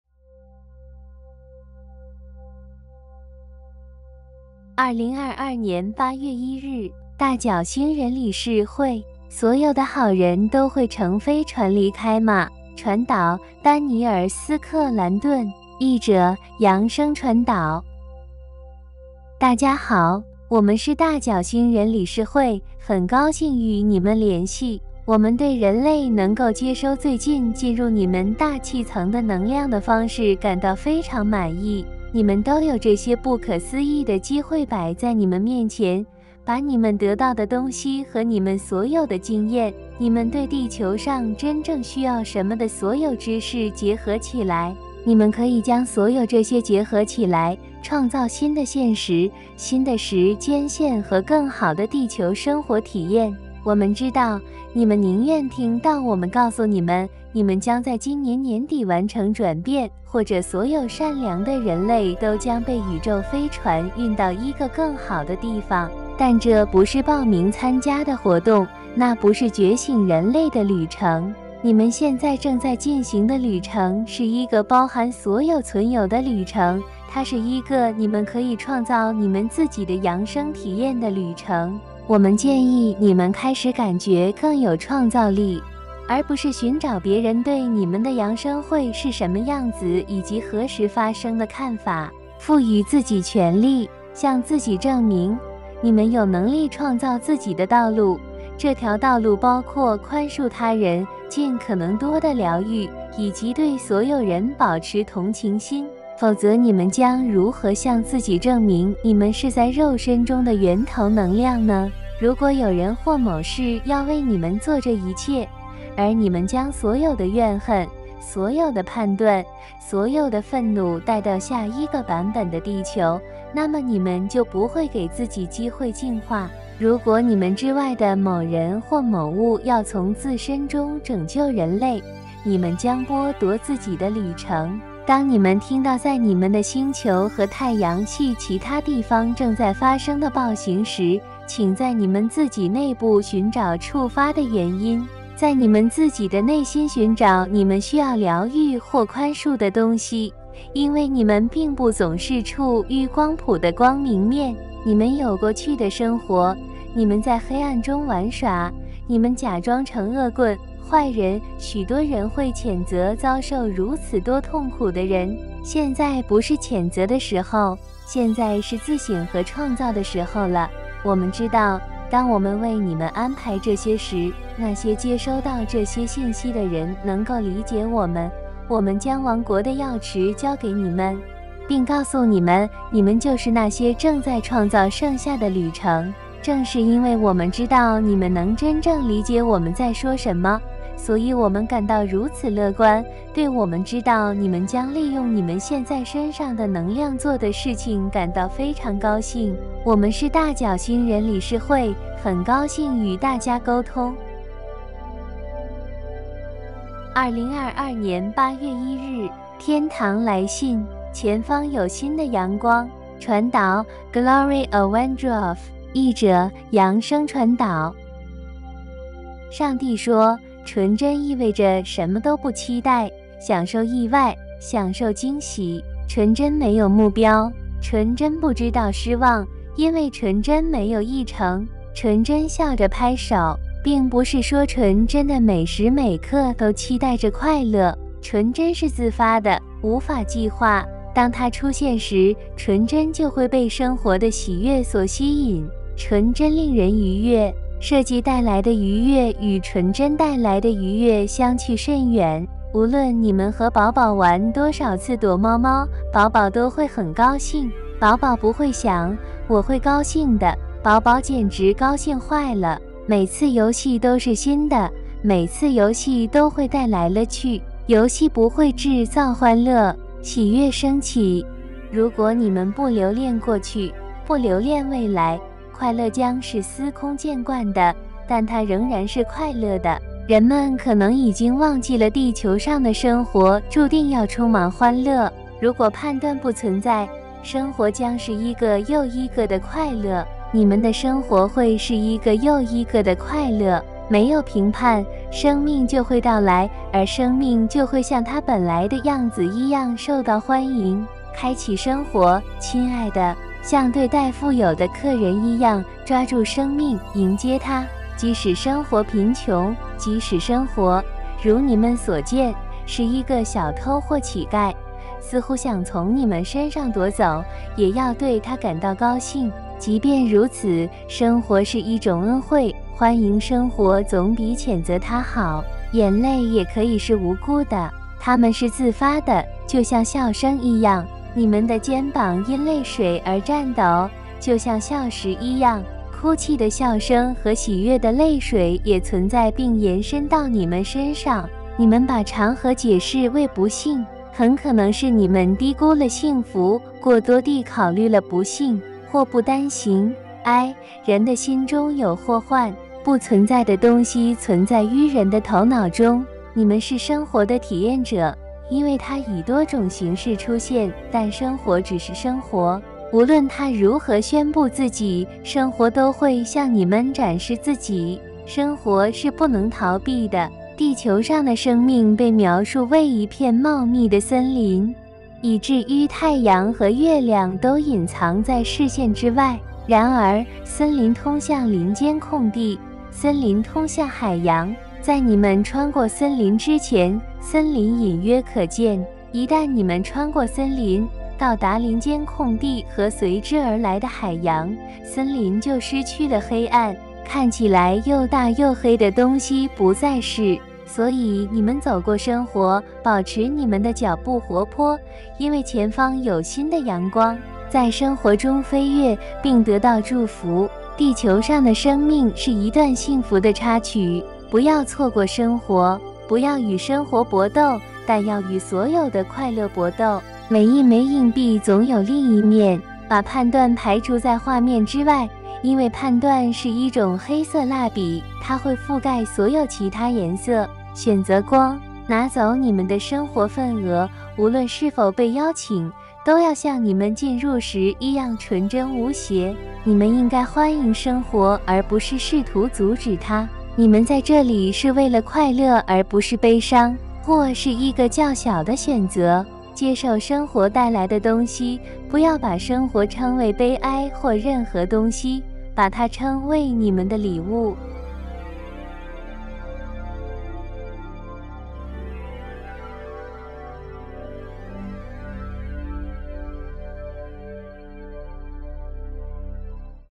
【大角星人理事會】《所有的好人都會乘飛船離開嗎？》【天堂來信】《前方有新的陽光》
00:00 開始
00:05 【大角星人理事會】《所有的好人都會乘飛船離開嗎？》
03:26 【天堂來信】《前方有新的陽光》

2022年8月1日【大角星人理事會】

《所有的好人都會乘飛船離開嗎？》

我們對人類能夠接收最近進入你們大氣層的能量的方式感到非常滿意。你們都有這些不可思議的機會擺在你們面前，把你們得到的東西和你們所有的經驗、你們對地球上真正需要什麼的所有知識結合起來。你們可以將所有這些結合起來，創造新的現實、新的時間線和更好的地球生活體驗。

我們知道，你們寧願聽到我們告訴你們，你們將在今年年底完成轉變，或者所有善良的人類都將被宇宙飛船運到一個更好的地方。但這不是報名參加的活動，那不是覺醒人類的旅程。你們現在正在進行的旅程是一個包含所有存有的旅程，它是一個你們可以創造你們自己的揚升體驗的旅程。

我們建議你們開始感覺更有創造力，而不是尋找別人對你們的揚升會是什麼樣子以及何時發生的看法。賦予自己權力。向自己證明，你們有能力創造自己的道路，這條道路包括寬恕他人、盡可能多地療愈，以及對所有人保持同情心。否則你們將如何向自己證明你們是在肉身中的源頭能量呢？如果有人或某事要為你們做這一切，而你們將所有的怨恨、所有的判斷、所有的憤怒帶到下一個版本的地球，那麼你們就不會給自己機會進化。

如果你們之外的某人或某物要從自身中拯救人類，你們將剝奪自己的旅程。當你們聽到在你們的星球和太陽係其他地方正在發生的暴行時，請在你們自己內部尋找觸發的原因。在你們自己的內心尋找你們需要療愈或寬恕的東西，因為你們並不總是處於光譜的光明面。

你們有過去的生活，你們在黑暗中玩耍，你們假裝成惡棍，壞人，許多人會譴責遭受如此多痛苦的人。現在不是譴責的時候。現在是自省和創造的時候了，我們知道，當我們為你們安排這些時，那些接收到這些信息的人能夠理解我們，我們將王國的鑰匙交給你們，並告訴你們，你們就是那些正在創造剩下的旅程。

正是因為我們知道你們能真正理解我們在說什麼，所以我們感到如此樂觀，對我們知道你們將利用你們現在身上的能量做的事情感到非常高興。

2022年8月1日【天堂來信】

《前方有新的陽光》

上帝說：

純真意味着什麼都不期待，享受意外，享受驚喜。

純真沒有目標。純真不知道失望，因為純真沒有議程。純真笑着拍手。

並不是說純真的每時每刻都期待着快樂。純真是自發的，無法計劃。當它出現時，純真就會被生活的喜悅所吸引。純真令人愉悅。設計帶來的愉悅與純真帶來的愉悅相去甚遠。

無論你們和寶寶玩多少次躲貓貓，寶寶都會很高興。寶寶不會想：“我會高興的。” 寶寶簡直高興壞了。每次遊戲都是新的，每次遊戲都會帶來樂趣。遊戲不會製造歡樂。喜悅升起。

如果你們不留戀過去，不留戀未來，快樂將是司空見慣的，但它仍然是快樂的。

人們可能已經忘記了地球上的生活注定要充滿歡樂。

如果判斷不存在，生活將是一個又一個的快樂。你們的生活會是一個又一個的快樂。沒有評判，生命就會到來，而生命就會像它本來的樣子一樣受到歡迎。

開啟生活，親愛的。像對待富有的客人一樣，抓住生命，迎接它。即使生活貧窮，即使生活，如你們所見，是一個小偷或乞丐，似乎想從你們身上奪走，也要對它感到高興。

即便如此，生活是一種恩惠，歡迎生活總比譴責它好。

眼淚也可以是無辜的。它們是自發的，就像笑聲一樣。你們的肩膀因淚水而顫抖，就像笑時一樣。哭泣的笑聲和喜悅的淚水也存在並延伸到你們身上。

你們把場合解釋為不幸。很可能是你們低估了幸福，過多地考慮了不幸。禍不單行。唉，人的心中有禍患。不存在的東西存在於人的頭腦中。

你們是生活的體驗者，因為它以多種形式出現，但生活只是生活。無論它如何宣布自己，生活都會向你們展示自己。生活是不能逃避的。

地球上的生命被描述為一片茂密的森林，以至於太陽和月亮都隱藏在視線之外。然而，森林通向林間空地，森林通向海洋。在你們穿過森林之前，森林隱約可見。一旦你們穿過森林，到達林間空地和隨之而來的海洋，森林就失去了黑暗。看起來又大又黑的東西不再是。所以你們走過生活，保持你們的腳步活潑，因為前方有新的陽光。

在生活中飛躍，並得到祝福。

地球上的生命是一段幸福的插曲。不要錯過生活。不要與生活搏鬥，但要與所有的快樂搏鬥。每一枚硬幣總有另一面。把判斷排除在畫面之外，因為判斷是一種黑色蠟筆，它會覆蓋所有其他顏色。選擇光。拿走你們的生活份額，無論是否被邀請，都要像你們進入時一樣純真無邪。

你們應該歡迎生活而不是試圖阻止它。你們在這裡是為了快樂而不是悲傷。禍是一個較小的選擇。接受生活帶來的東西，不要把生活稱為悲哀或任何東西，把它稱為你們的禮物。